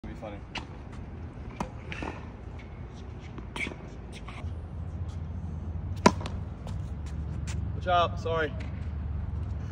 Trick shot Serve Expectation Vs Reality
Should I try this in a match ?!

Video with famous tennis coach Dylan on  @FelixMischker.Official